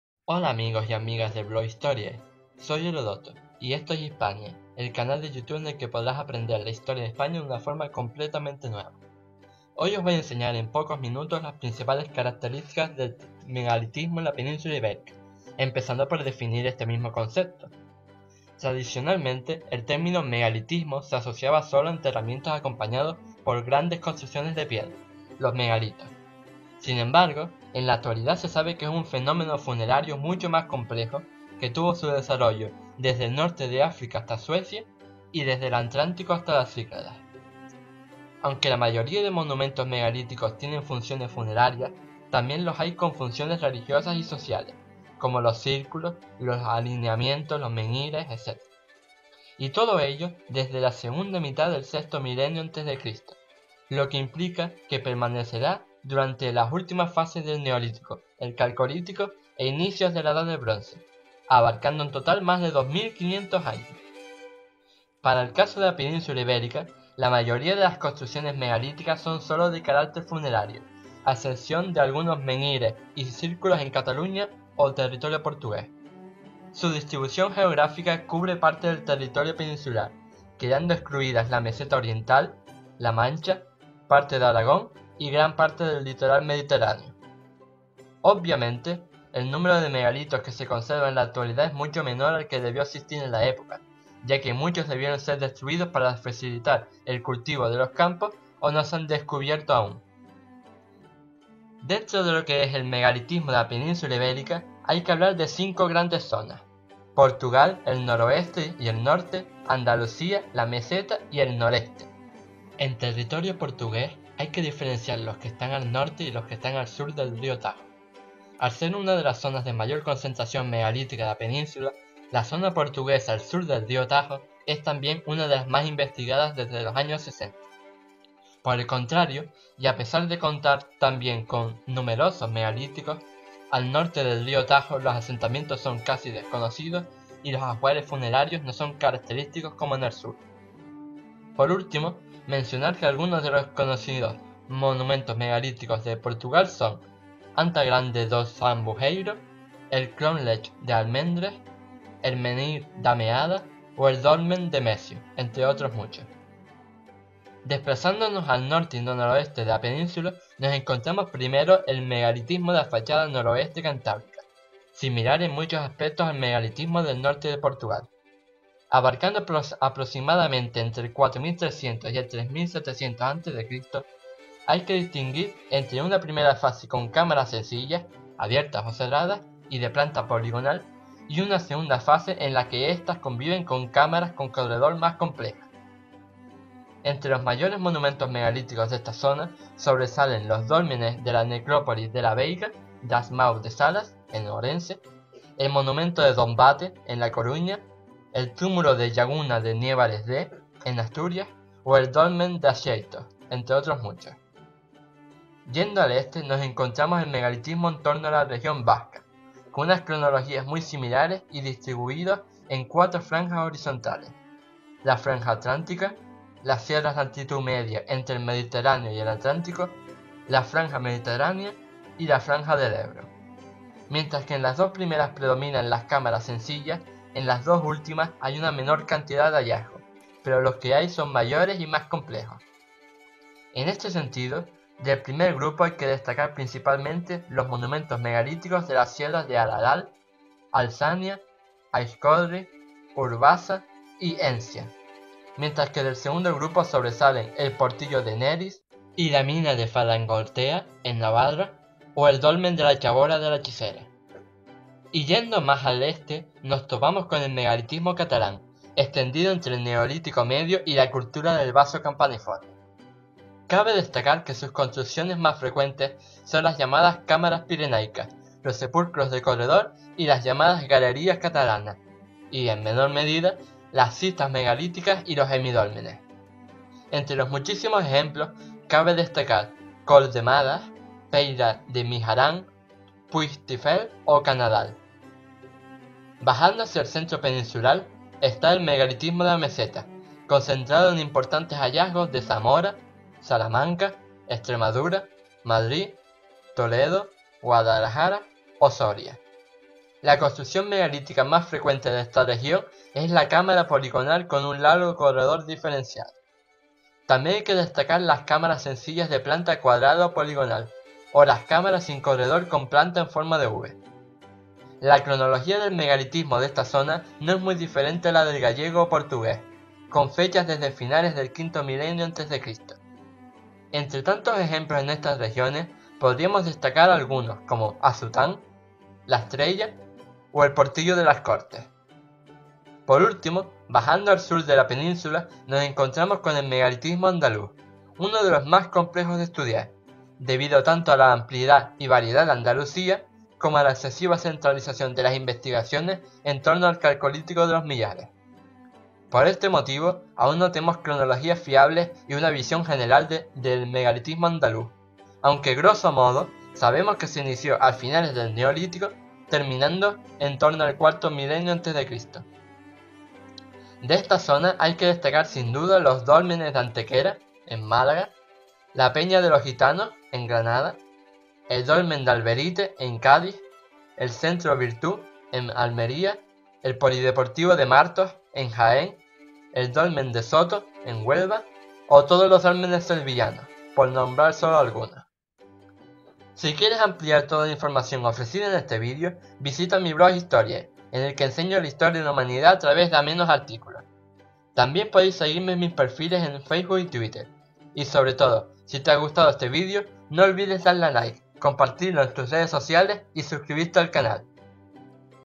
Megalitismo en la Península Ibérica
En este sexto vídeo te enseño las principales características del megalitismo en la península Ibérica, distinguiendo entre sus principales zonas de desarrollo. Si quieres ampliar toda la información ofrecida en este vídeo, visita mi blog Historiae, en el que enseño la Historia de la Humanidad a través de amenos artículos. También podéis seguirme en mis perfiles en Facebook y Twitter.